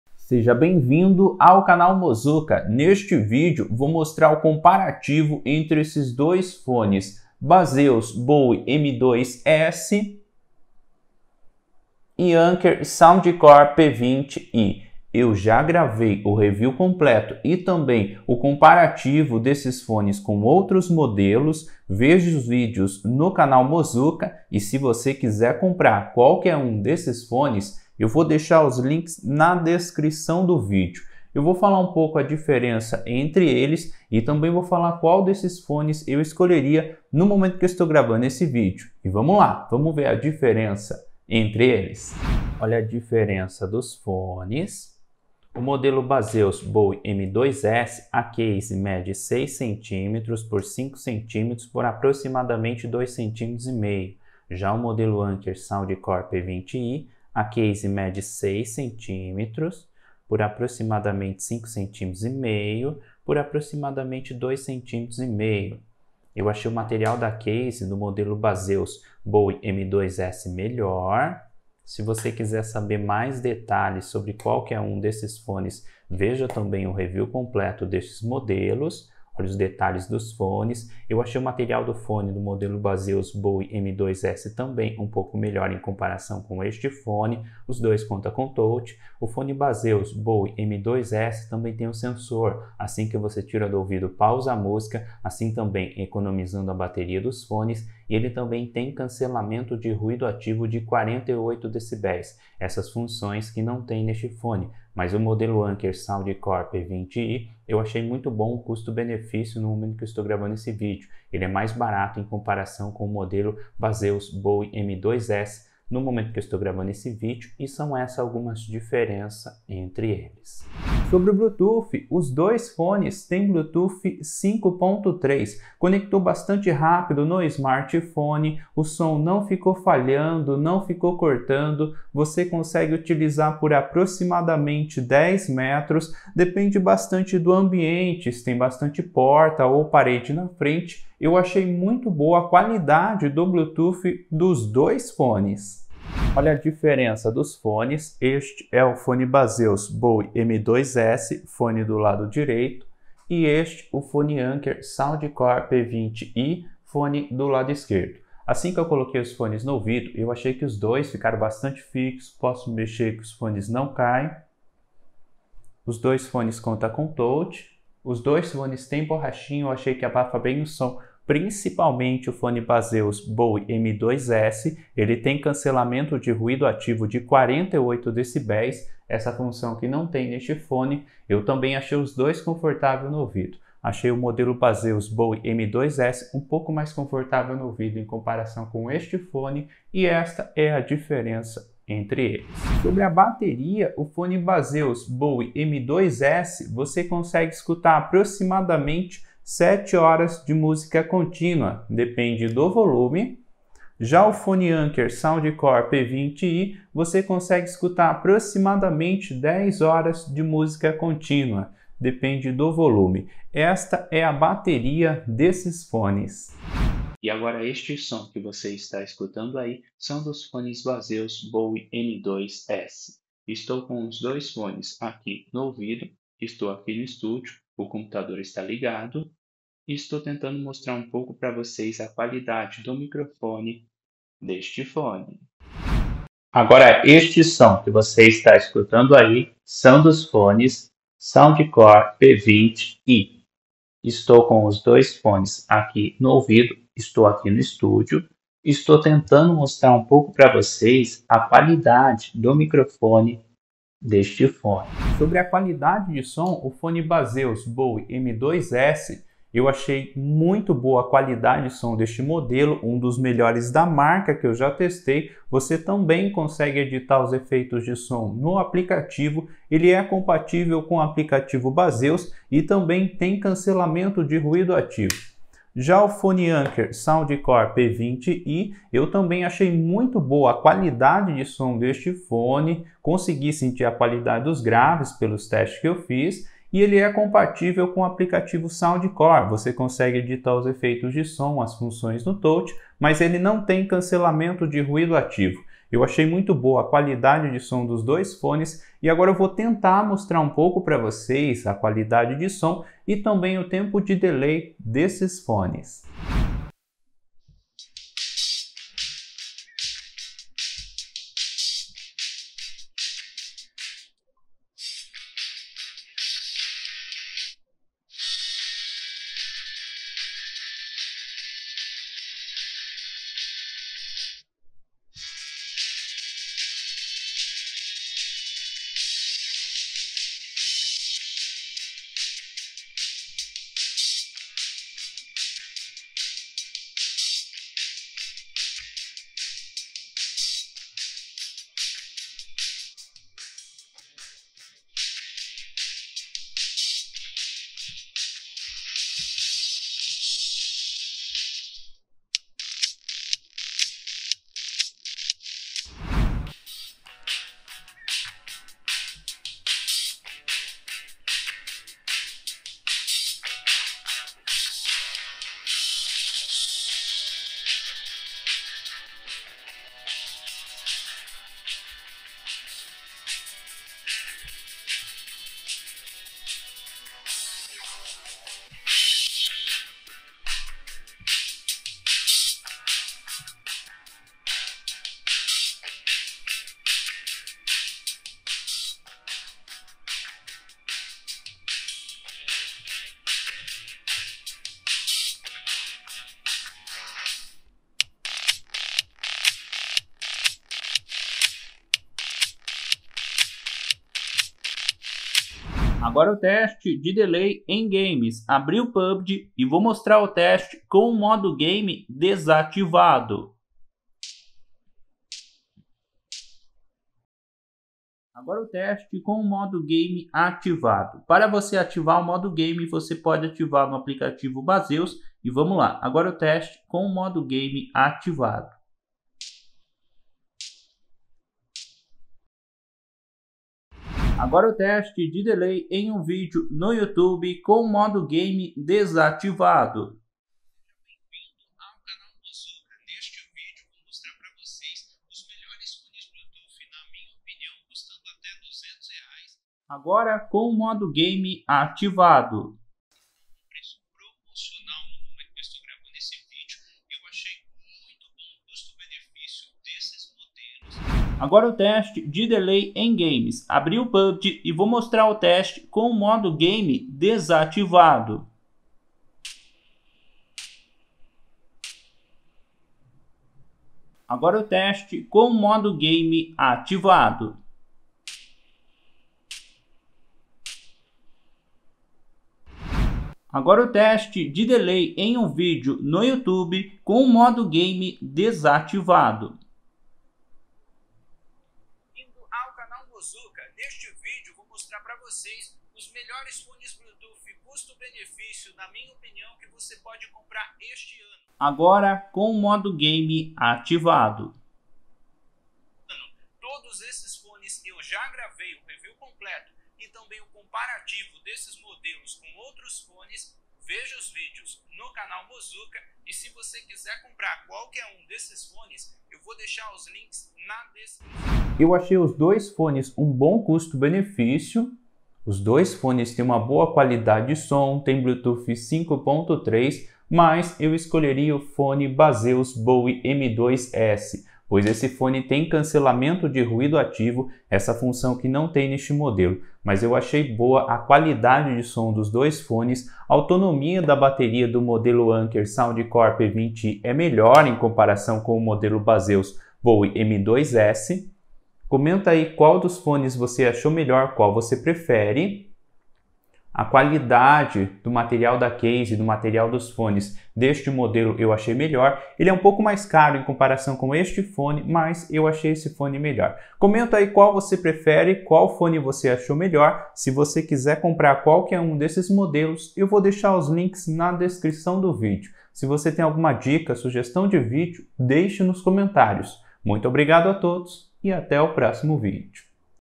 Baseus Bowie M2S vs Anker P20i Comparativo!! Teste de SOM, MIC e DELAY, qual é melhor?
Anker Soundcore P20i na Promoção

(00:00) Introdução
(00:31) Diferença entre os fones
(01:22) Bluetooth
(01:59) Como fica no ouvido
(02:54) Bateria
(03:29) Teste de Microfone Baseus Bowie M2S
(03:48) Teste de Microfone Haylou Moripods ANC
(04:04) Qualidade de Som
(04:46) Teste de Som Baseus Bowie M2S
(05:36) Teste de Som Haylou Moripods ANC
(06:00) Teste de Delay Baseus Bowie M2S
(06:55) Teste de Delay Haylou Moripods ANC
(07:49) Qual fone é melhor?